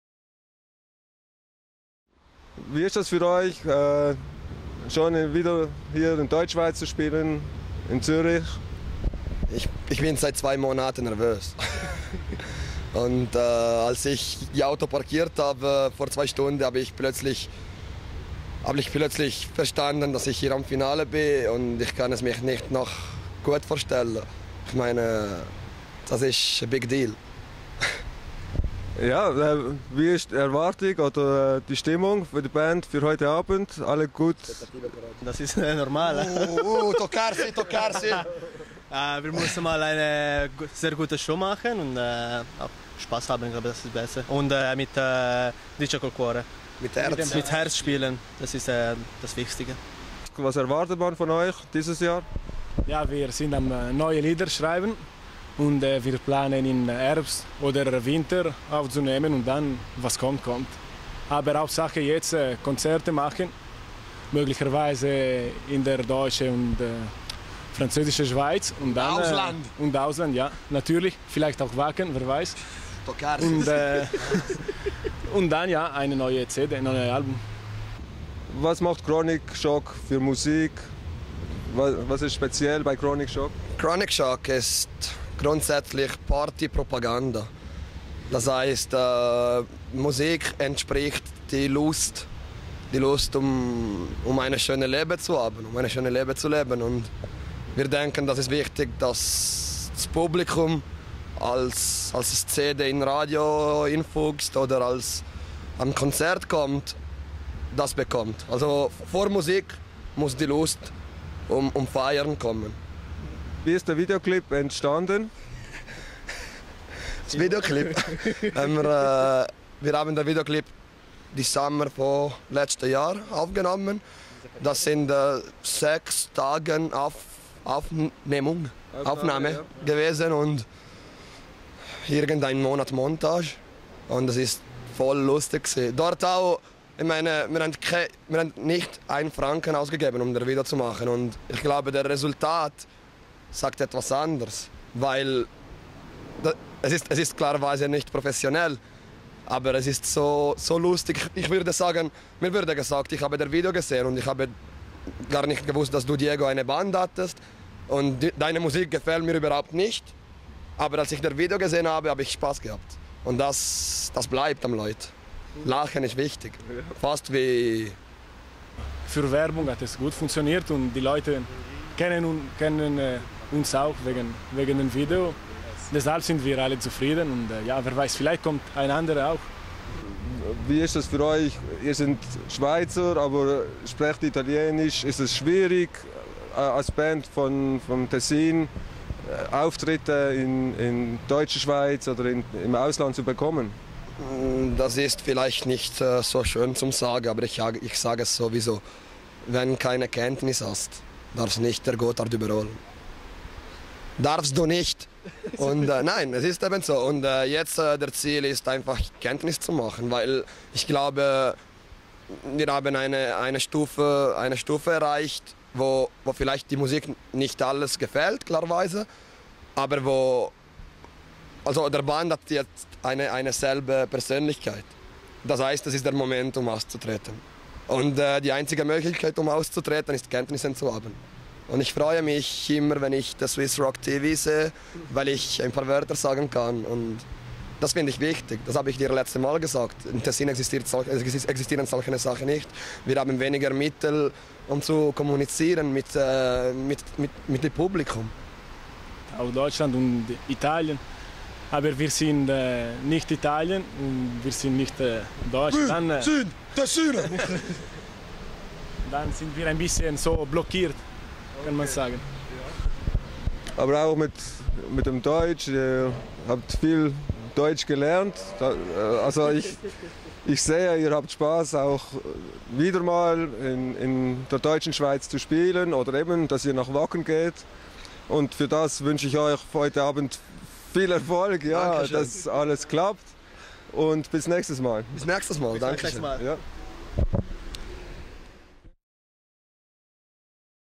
Chronic Shock Interview vor dem Dynamo
Interview mit Chronic Shock am Swiss Metal-Battle Finale vor dem Dynamo in Zürich, 2011.